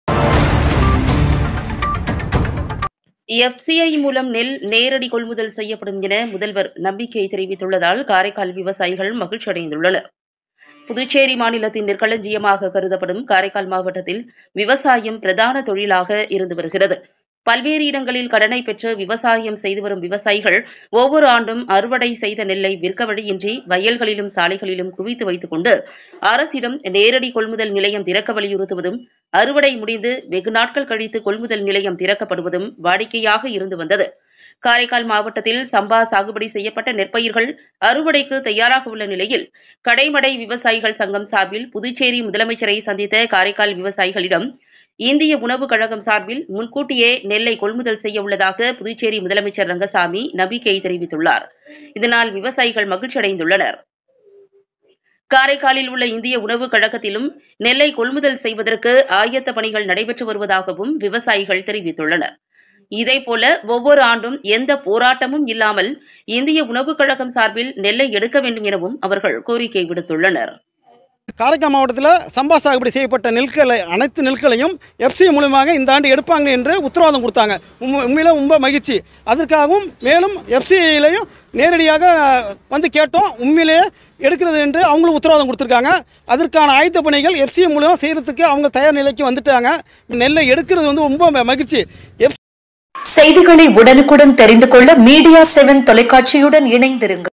எப்.சி.ஐ மூலம் நெல் நேரடி கொள்முதல் விவசாயிகள் மகிழ்ச்சி..!
எப்.சி.ஐ மூலம் நெல் நேரடி கொள்முதல் செய்யப்படும் என முதல்வர் நம்பிக்கை. காரைக்கால் விவசாயிகள் மகிழ்ச்சி.

     புதுச்சேரி மாநிலத்தின் நெற்களஞ்சியமாக கருதப்படும் காரைக்கால் மாவட்டத்தில் விவசாயம் பிரதான தொழிலாக இருந்து வருகிறது. பல்வேறு இடங்களில் கடனை பெற்று விவசாயம் செய்து வரும் விவசாயிகள் ஒவ்வொரு ஆண்டும் அறுவடை செய்த நெல்லை விற்க வழியின்றி வயல்களிலும் சாலைகளிலும் குவித்து வைத்துக் கொண்டு அரசிடம் நேரடி கொள்முதல் நிலையம் திறக்க வலியுறுத்துவதும் அறுவடை முடிந்து வெகு நாட்கள் கழித்து கொள்முதல் நிலையம் திறக்கப்படுவதும் வாடிக்கையாக இருந்து வந்தது.  காரைக்கால் மாவட்டத்தில் சம்பா சாகுபடி செய்யப்பட்ட நெற்பயிர்கள் அறுவடைக்கு தயாராக உள்ள நிலையில் கடைமடை விவசாயிகள் சங்கம் சார்பில் புதுச்சேரி முதலமைச்சரை சந்தித்த காரைக்கால் விவசாயி விவசாயிகளிடம் இந்திய உணவுக் கழகம் சார்பில் முன்கூட்டியே நெல்லை கொள்முதல் செய்ய உள்ளதாக புதுச்சேரி முதலமைச்சர் ரங்கசாமி நம்பிக்கை தெரிவித்துள்ளதால் விவசாயிகள் மகிழ்ச்சி அடைந்துள்ளனர். காரைக்காலில் உள்ள இந்திய உணவுக் கழகத்திலும் நெல்லை கொள்முதல் செய்வதற்கு ஆயத்த பணிகள் நடைபெற்று வருவதாகவும் விவசாயிகள் தெரிவித்துள்ளனர். இதே போல ஒவ்வொரு ஆண்டும் எந்த போராட்டமும் இல்லாமல் இந்திய உணவுக் கழகம் சார்பில் நெல்லை எடுக்க வேண்டும் எனவும் அவர்கள் கோரிக்கை விடுத்துள்ளனர்.